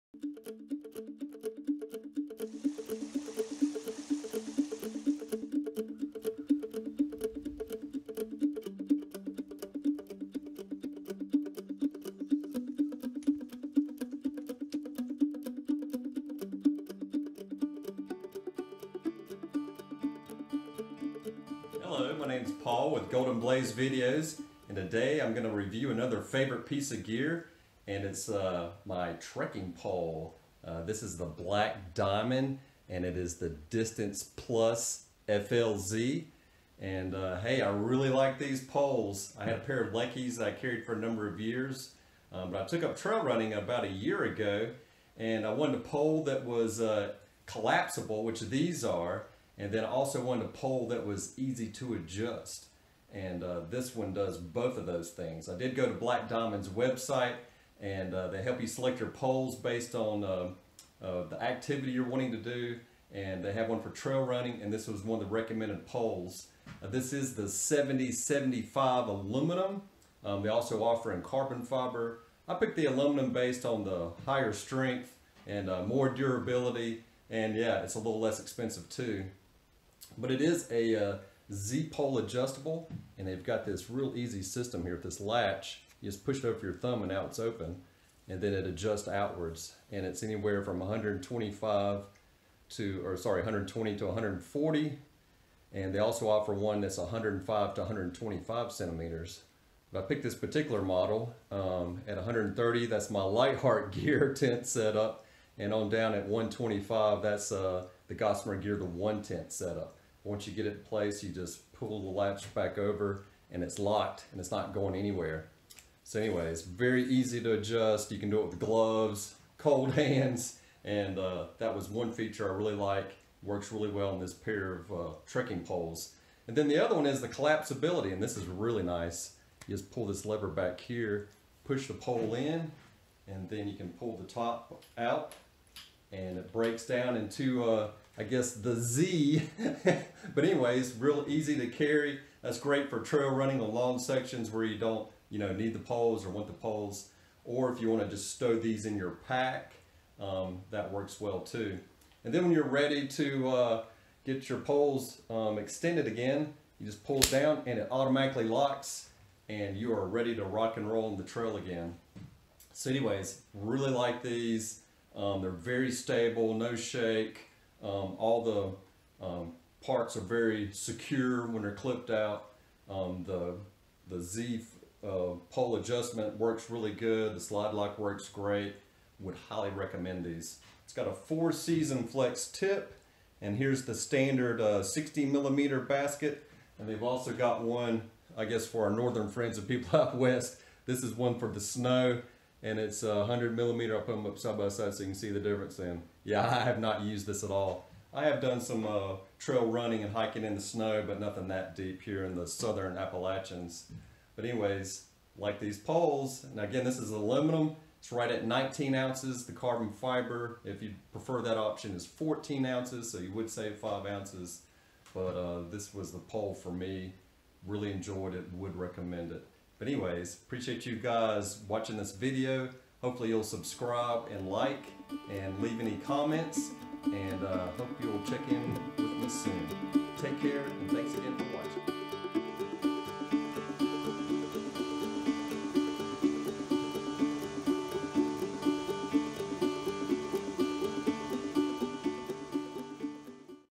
Black Diamond Distance Plus FLZ Trekking Poles Review
Black Diamond Distance Plus FLZ Trekking Poles Review.  These poles are light weight, super strong, Flick Lock easy length adjustment and have quick break down Z-pole technology for collapsing on the move.  Ideal for trail running and backpacking, the 7030 aluminum is strong, durable and very stable.  The quick and easy length adjustment works great for setting up ultralight tents.  They also break down easily into the Z-pole configuration while on the move, and snap back into extended position even faster.  After 6 months use, my only regret is not buying these sooner.

Chapters
0:00 Intro
0:30 Distance Plus FLZ
0:45 Pole Selection
1:20 Flick Lock Adjustment
2:06 Z-Pole Technology
3:00 Basket Options
3:26 Aluminum vs Carbon Fiber